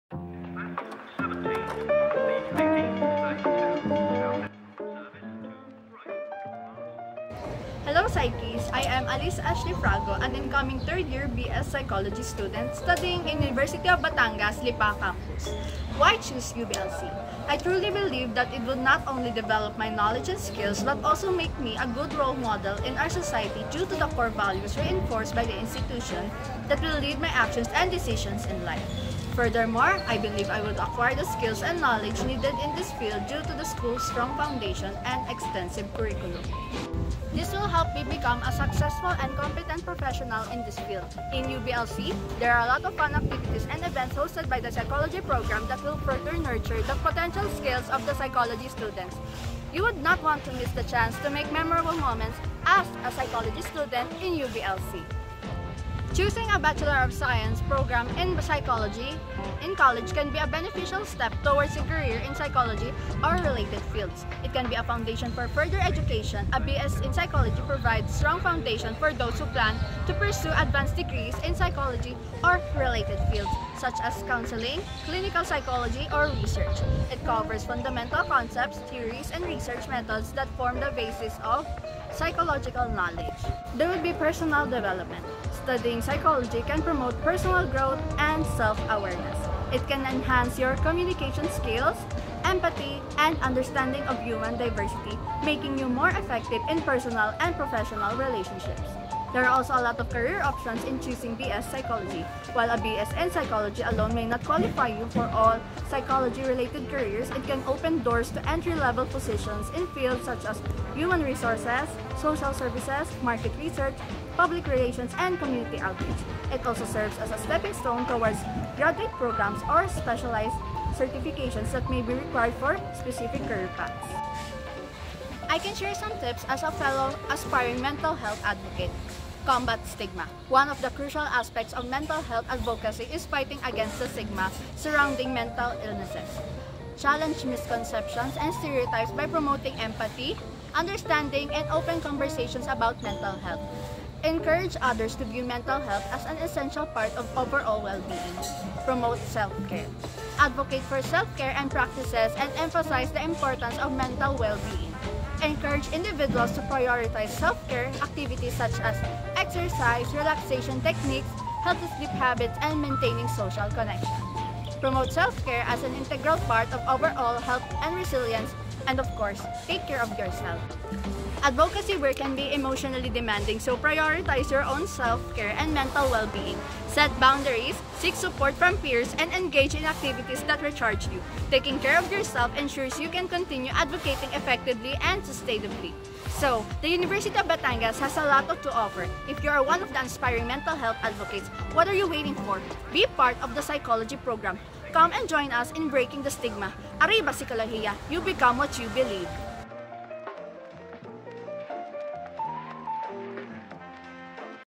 Psych Diaries: Peering into the Stories of Psychology Students in UBLC - Alyssa Ashley Frago
Are you curious about how the human mind shapes our behaviors, emotions, and thoughts? Do you have a passion for understanding what drives us to do the things we do? If so, a psychology program might be the perfect path for you! Join Ms. Alyssa Ashley Frago in unraveling the complexities of human nature at the University of Batangas Lipa Campus and be part of the Psychology community!